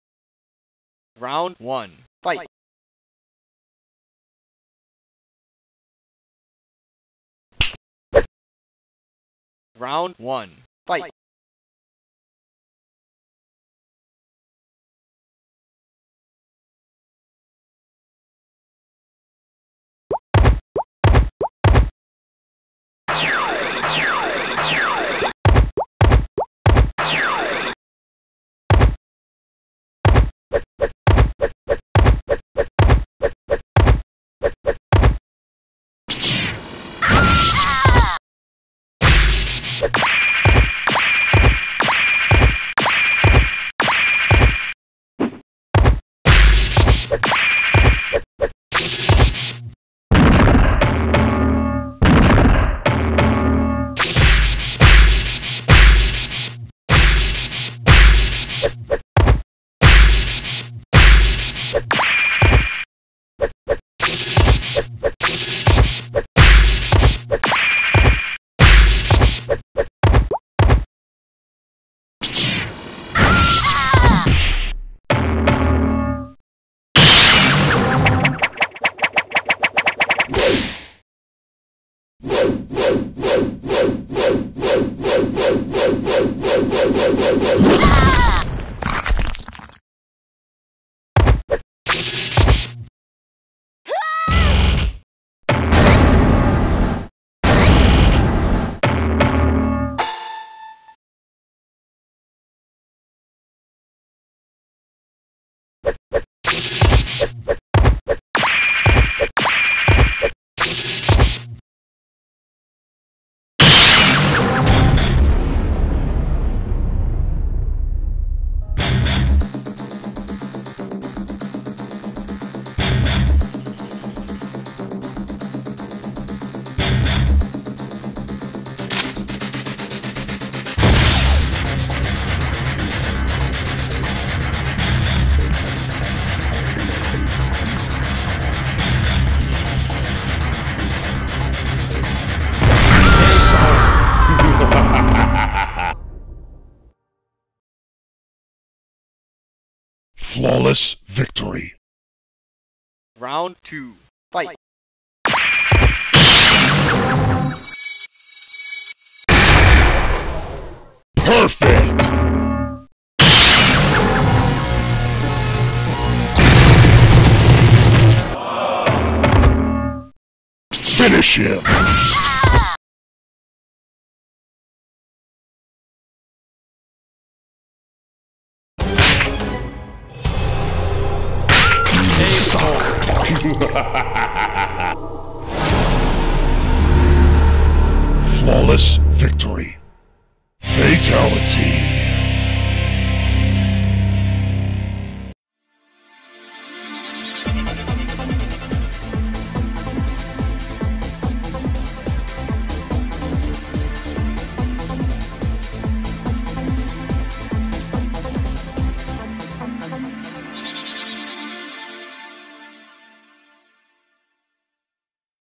A little update to my self insert
Walking animations, Dash animation, jump animations has been changed, Victory pose has been changed, added Luna Martinez to the intro, and the laser beam has been changed.